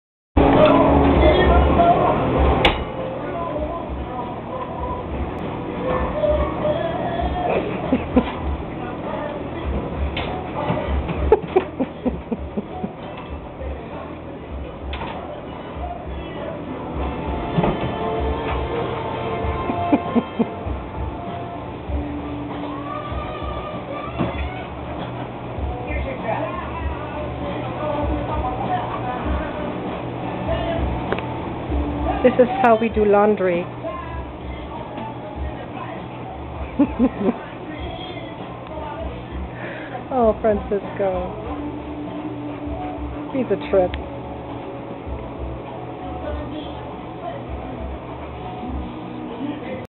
Francisco.MOV
Francisco helping Katherine with the laundry. Well - helping is probably not the right word.